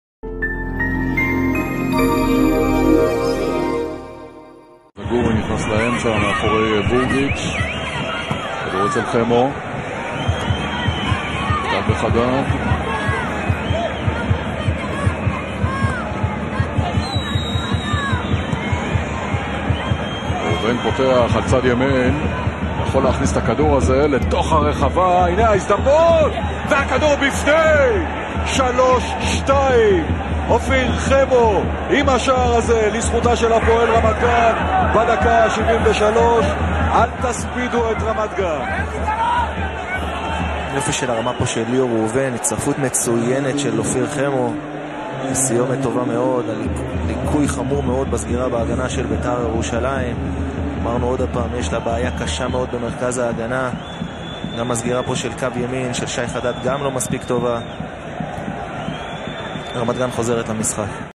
בית"ר ירושלים - הפועל רמת גן - השער של חמו
beitar jerusalem - hapoel ramat gan - ofir hemo's goal
משום מקום האורדונים חזרו למשחק. אופיר חמו שנכנס כמחליף לפני עשר דקות הגיע ראשון לכדור רוחב והמשיך מהאוויר לרשת של הרוש.